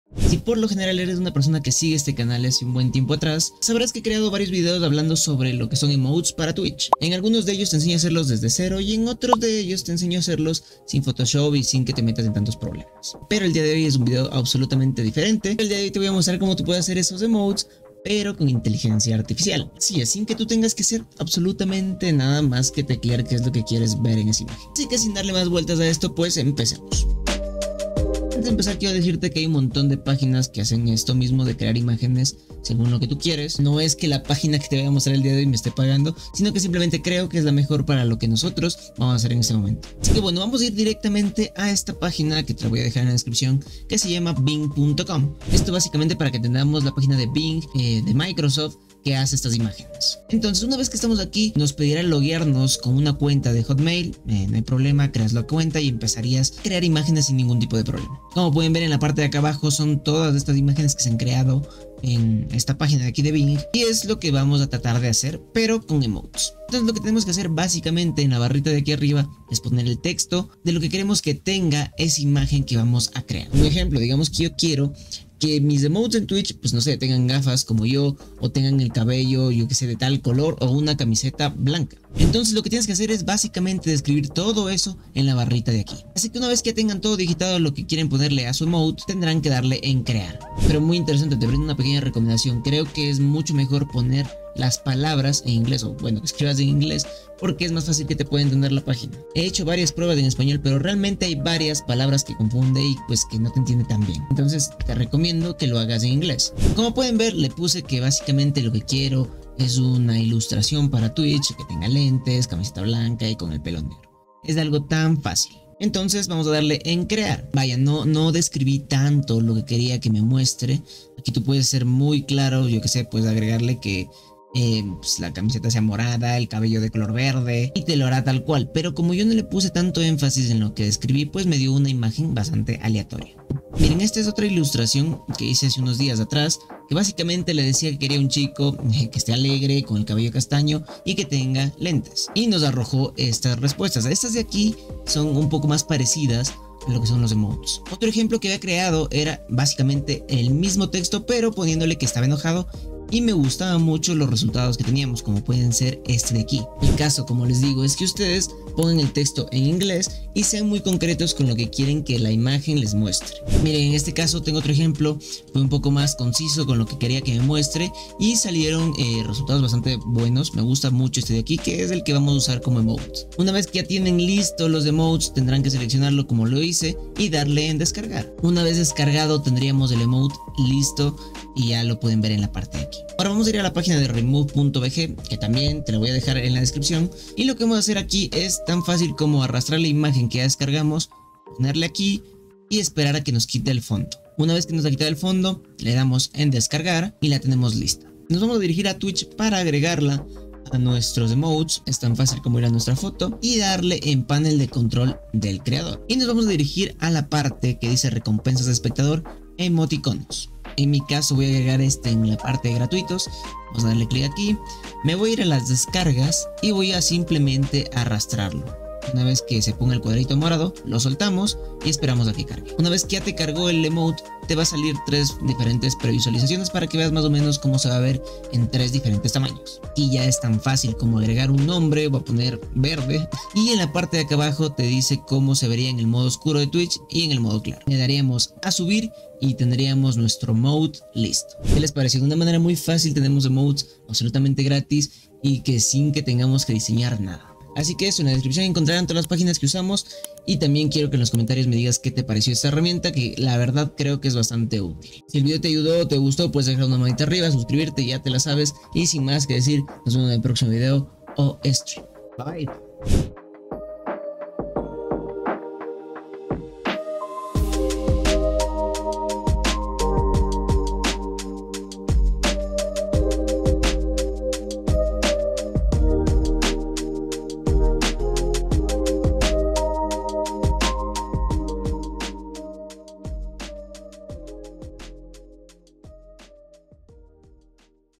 ✔️UTILIZA LA IA PARA CREAR EMOTES PARA TU CANAL DE TWITCH (GRATIS )
En este video vamos a abordar el tema de como hacer emotes para Twitch con inteligencia artificial. Esta manera de hacer emotes es bastante sencilla, y permite que el usuario se muestre en forma distinta dependiendo el estado de ánimo que esté experimentando. Por ejemplo, puede mostrarse agresivo cuando está enojado, o relajado cuando está feliz.

Codigo de creador Epic: ANDRESSEBASTIANN